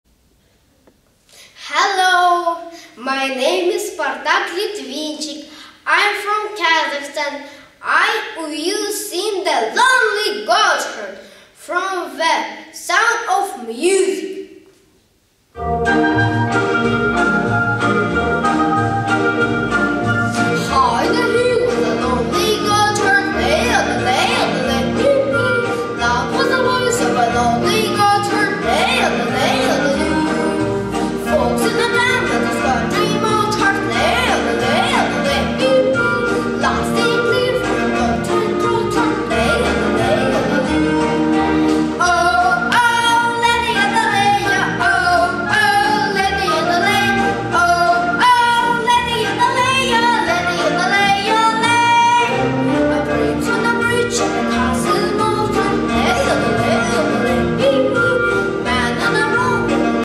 OHMTEC Contest 2021 - E - Litvinchik Spartak
Hammerstein International youth solo contest
Spartak Litvinchik 7 years old
Almaty, Kazakhstan